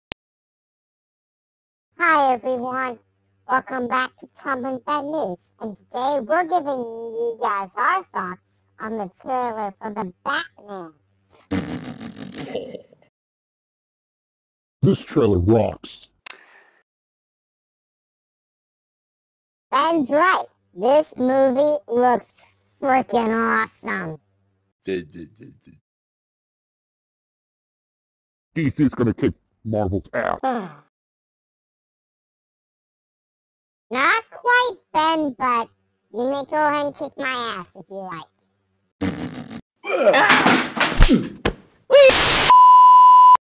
Tom And Ben Talk About The Teaser Trailer For The Batman
In this video, Tom and Ben talk about the teaser trailer for The Batman.